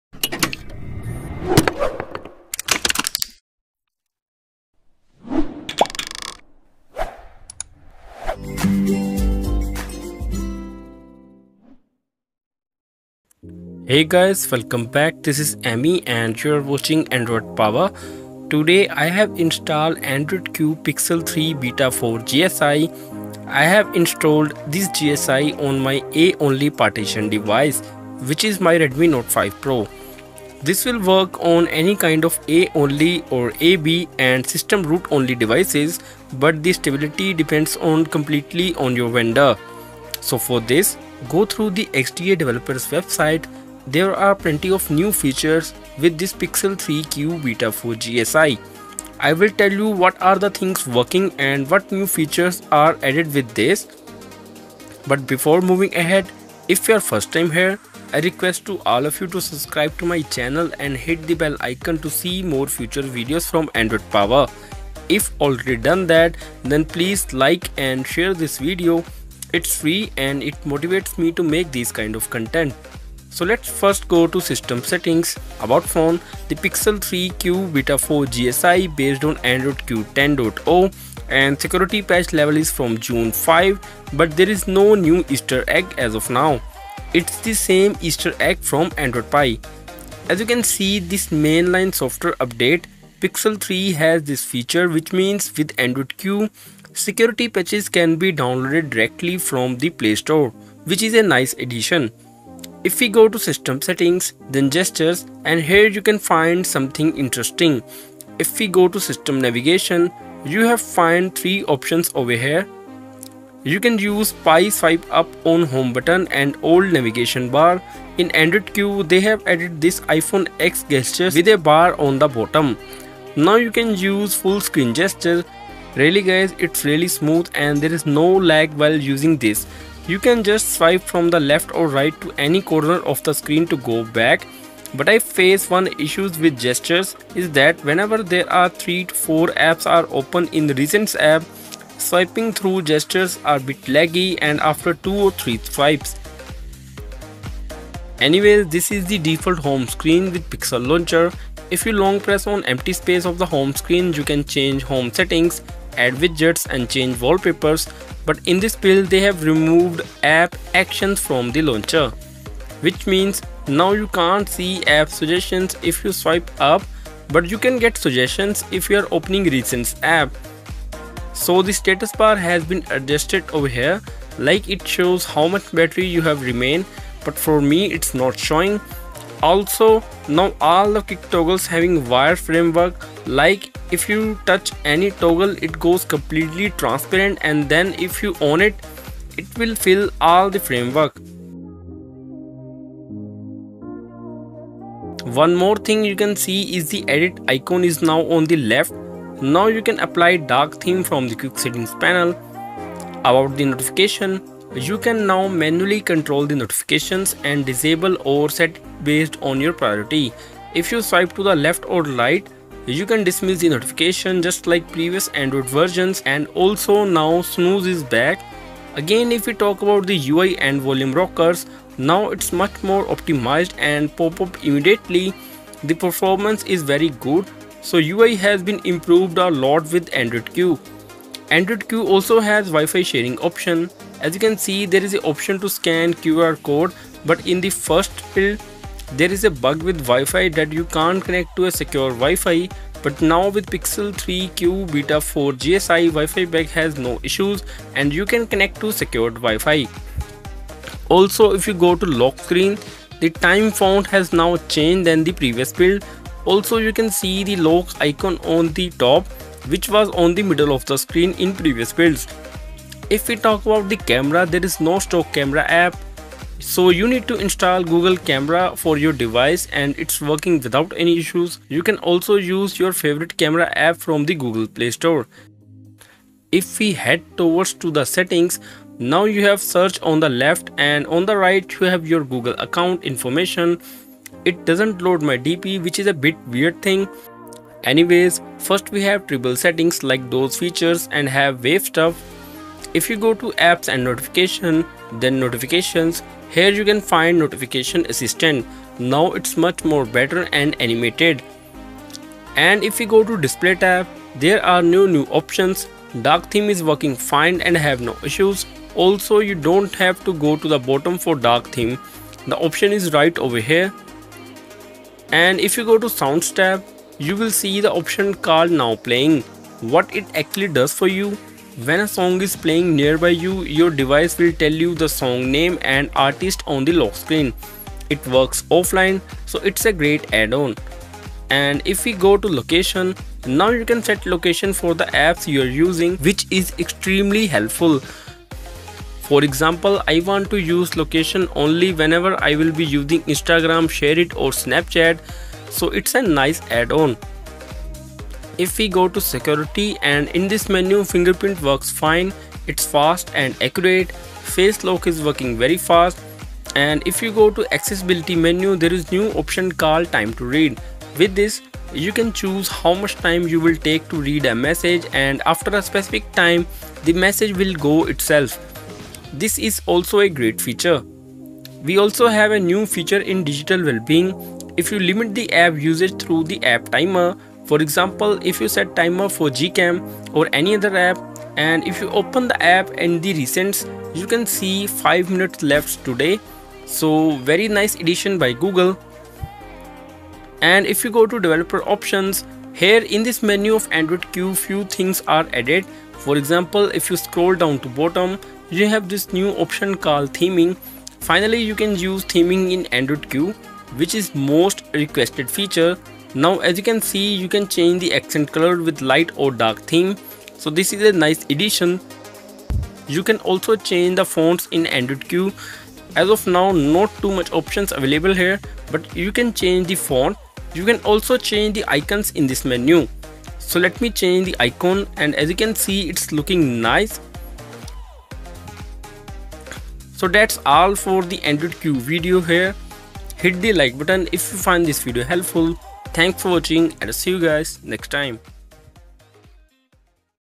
Pixel Q Beta 4 (Android 10) On Any Phone Review
Big thanks to Erfanoabdi for keeping up with GSI as soon as they popup.These are only for testing not suitable for daily use.

If Any One want to help this Channel By Donating: PAYPAL

☞Tripod m using

☞my Mic

☞my mount

☞ My primary phone